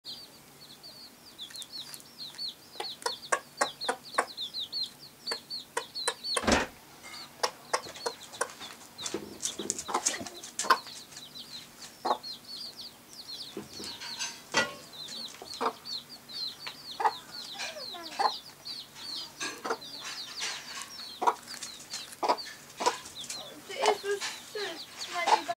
Kylleryller - kycklisar
Elviira matar senaste kullen